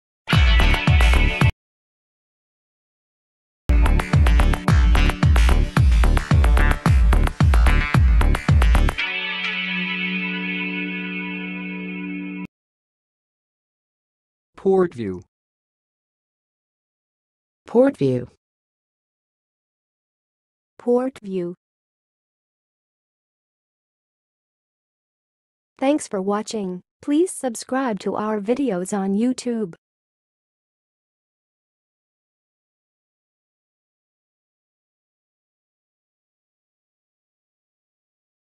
POORTVIEW - How to pronounce it?
POORTVIEW - Don't mispronounce Poortview anymore in 2023

Poortview is a suburban area located in Johannesburg, South Africa.
It is known for its scenic beauty, with rolling hills and breathtaking views.
The name 'Poortview' is derived from the Afrikaans word 'poort', meaning gate or entrance, and refers to the area's location at the entrance of the picturesque Kloofpoort Valley.
Poortview is highly sought after for its tranquility and proximity to nature, making it a popular residential area.

Pronunciation:
To pronounce 'Poortview', you can say 'poort-view' or 'poort-vyoo'.

Test yourself by speaking the following examples:
1. I visited Poortview last weekend and hiked along the beautiful trails
2. The real estate market in Poortview is booming
3. There are several excellent schools in Poortview for families with children
4. We enjoyed a delicious meal at one of the restaurants in Poortview
5. The community in Poortview is very active and organizes various events throughout the year